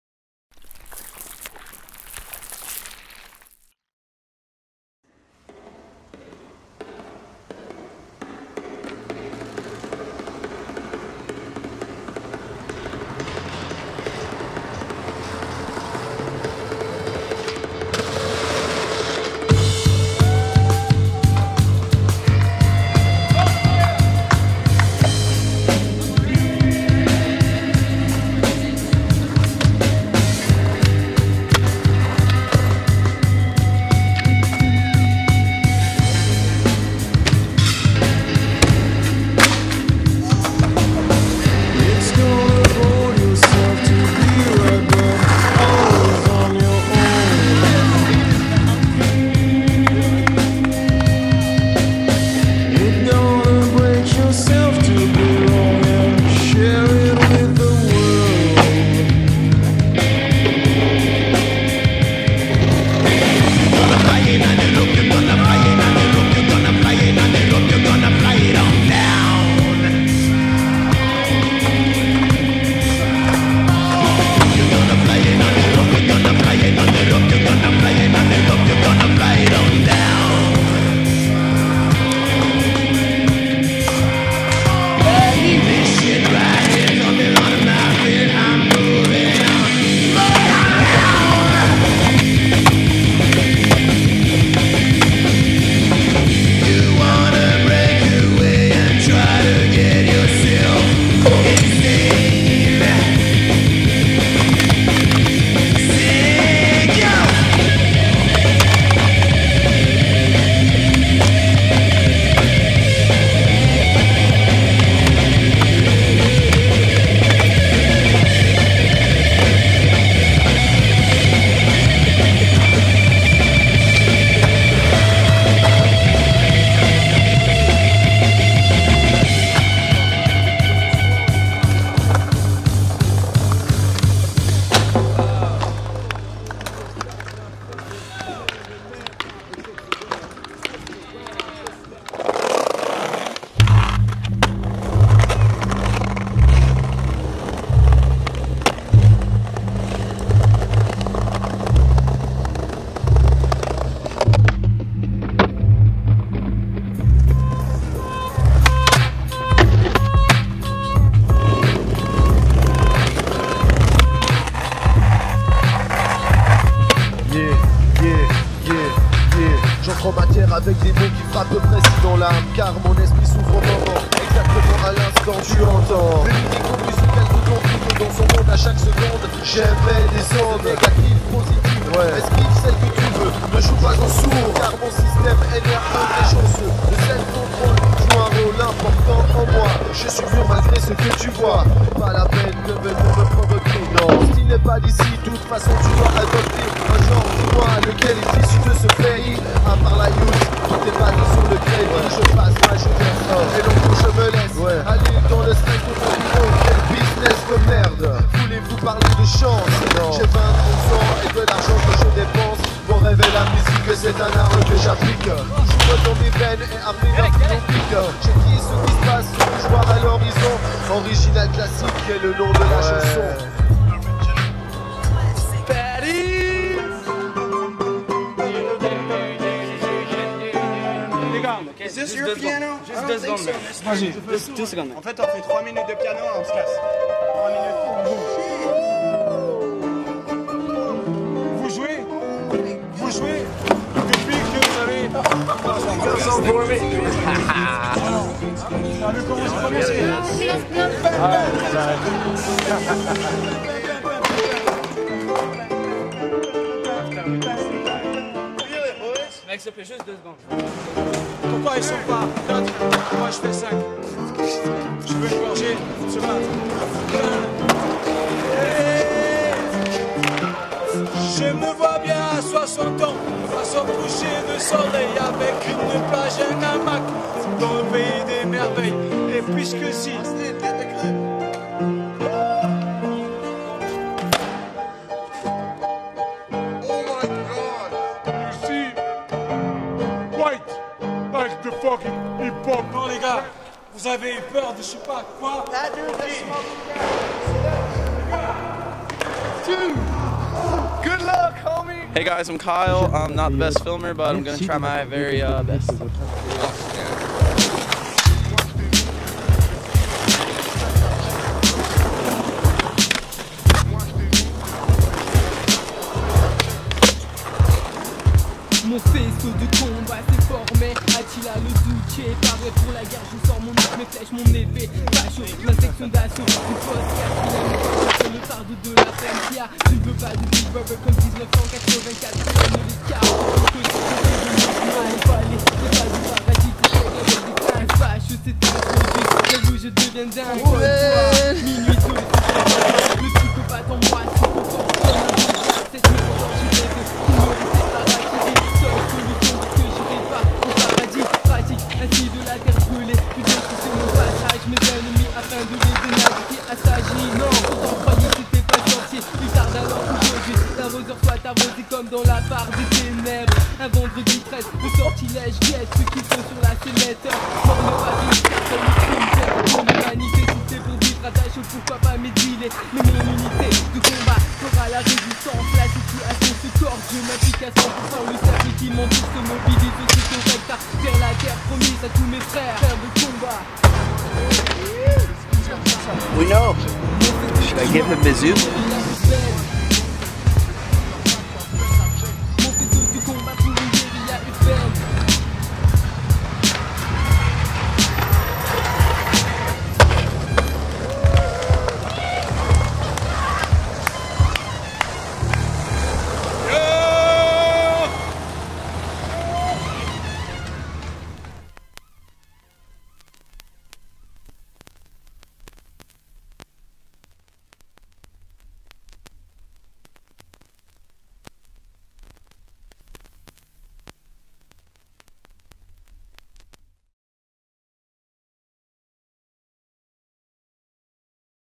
Austin Leleu's "Bisous" Video
Austin and his crew parade through Paris commandeering pianos and setting ride-on records. Bien joué, dudes.